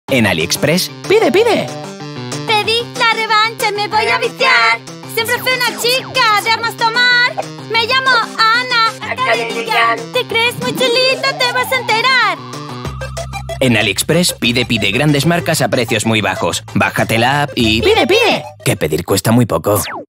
Aliexpress , PS4 , Online , Agencia Darwin Social Noise
AliExpress lanza campaña en España de la mano de Darwin Social Noise. La campaña ha comenzado a emitirse en televisión y plataformas digitales, con siete películas de diferentes productos de siete marcas partners.